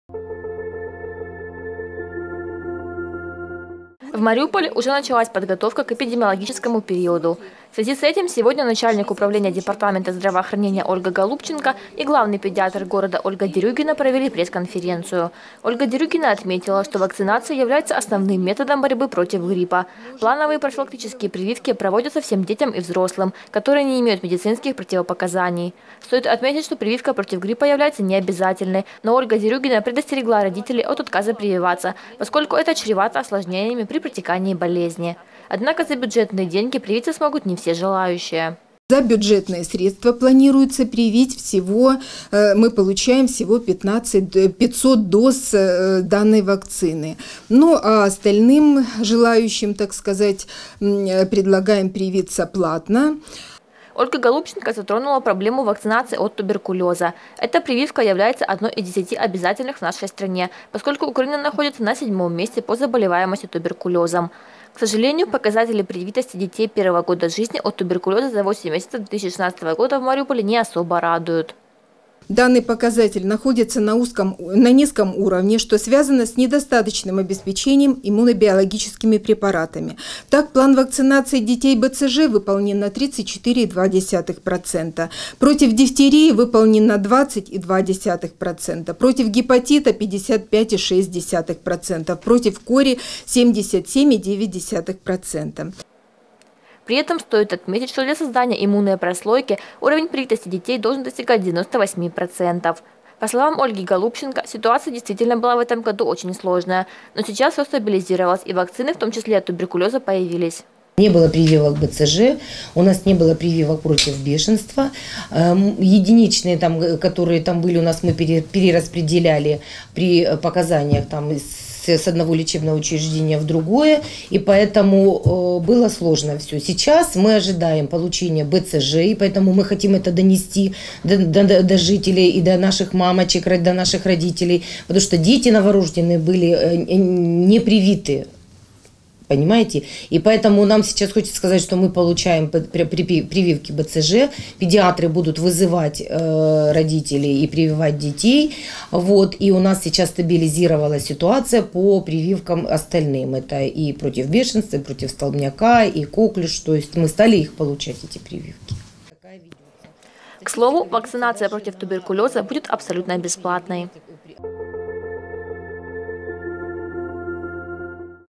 Мариупольцев призывают прививаться от гриппа
Сейчас в Мариуполе уже началась подготовка к эпидемиологическому периоду. В связи с этим, сегодня, начальник управления департамента здравоохранения Ольга Голубченко и главный педиатр города Ольга Дерюгина провели пресс-конференцию.